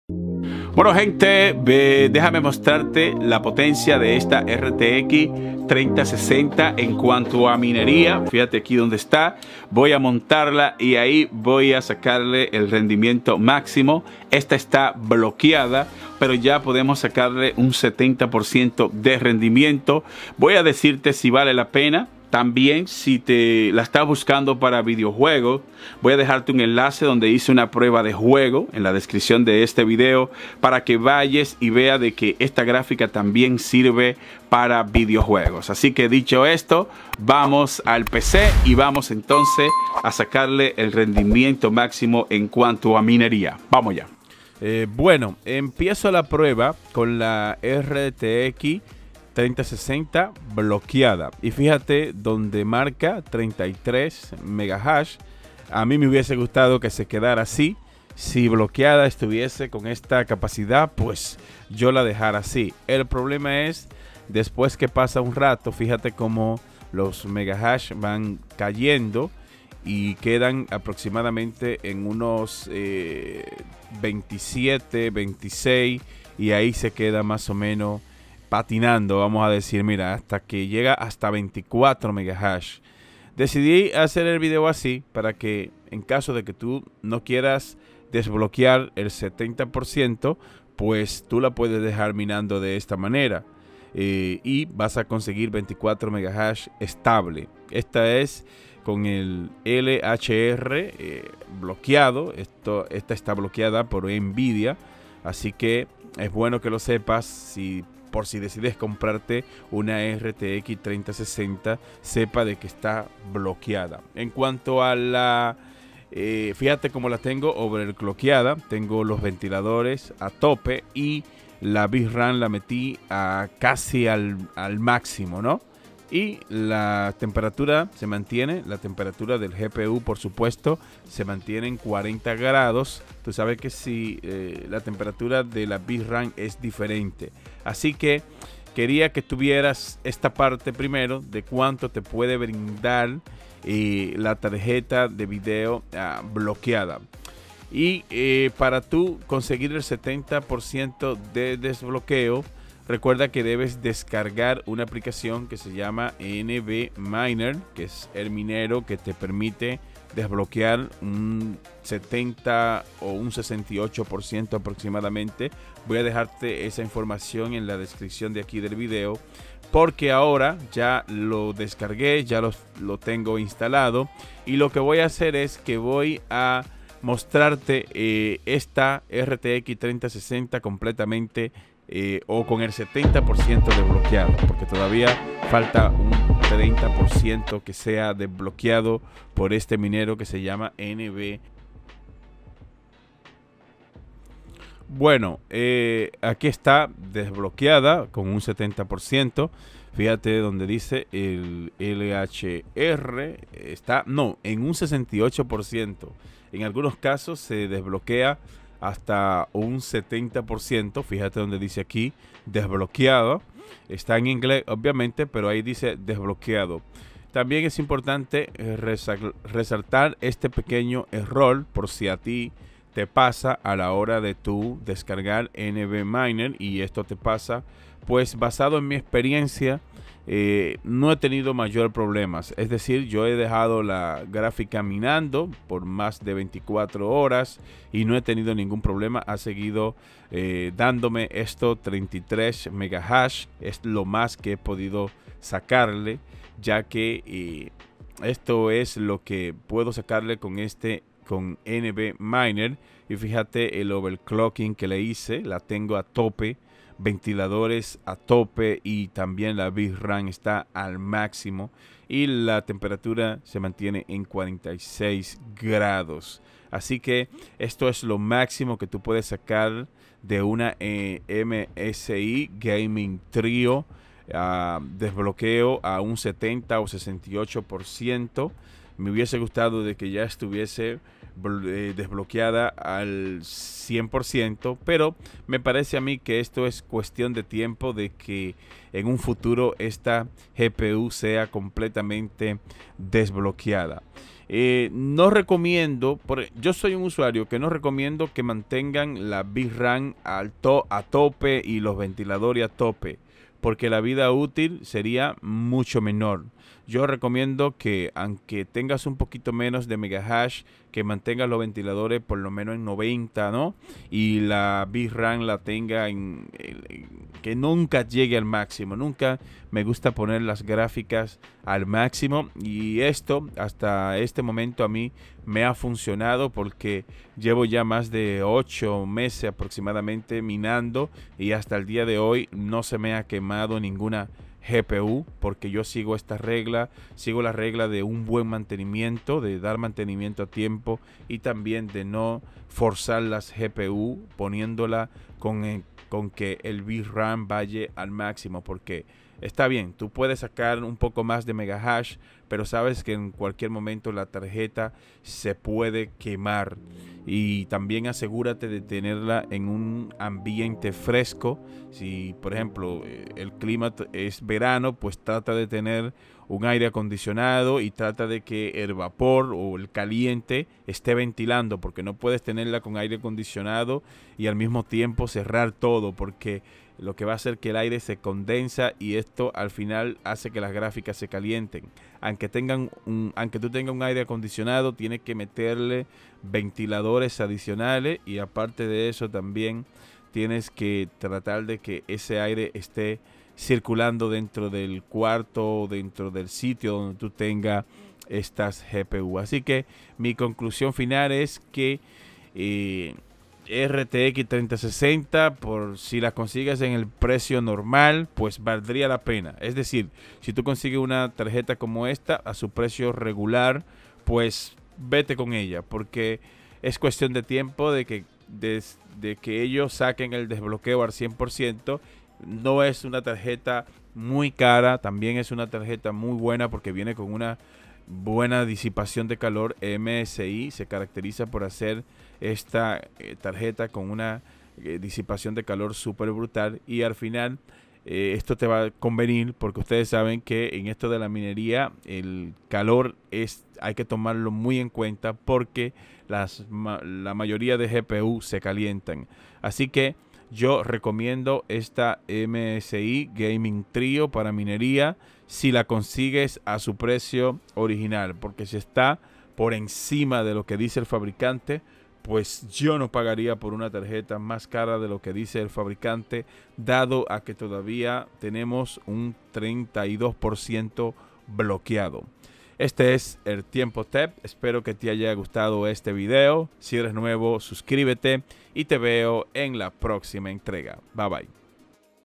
RTX 3060 MINANDO LHR DESBLOQUEADO 70%
MSI GeForce RTX 3060 GAMING X TRIO 12G Minando LHR Desbloqueado

Opción Compra
Otra Opción de compra

 Eso significa que si realiza un pedido utilizando el enlace, obtendremos una pequeña comisión sin costos adicionales para usted. ¡Gracias por tu apoyo!

0:00 Introducción
0:47 Prueba RTX 3060 Bloqueada
2:30 Desbloqueo 70% NBMiner
3:15 Prueba RTX 3060 desbloqueada
3:40 Información importante
4:55 Recomendación
6:41 Conclusión